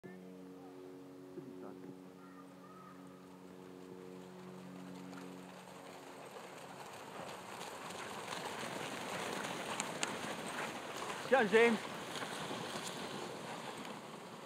Iditarod 2011-Corral Hill---James Bardoner
10 Miles down from the Iditarod restart in Willow, mushers hit their first big hill--Corral Hill. it's a good one.